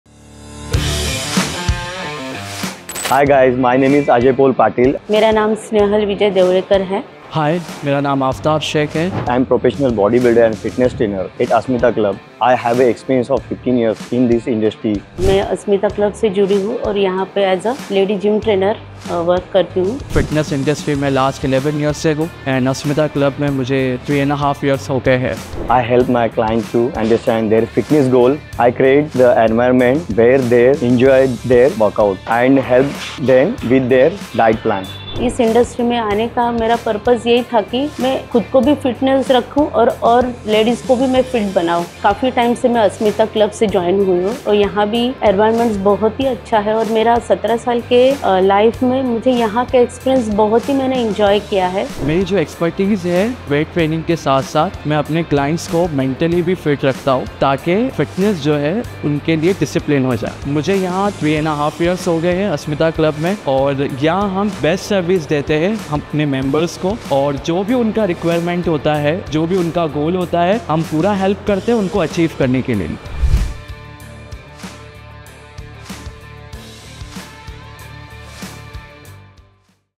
Certified Fitness Trainers
Why does AsmitA Club feel like home? ☺️

Hear from our amazing certified trainers who make every workout count!

Ready to join the family that’s trained to help you carve the best in you?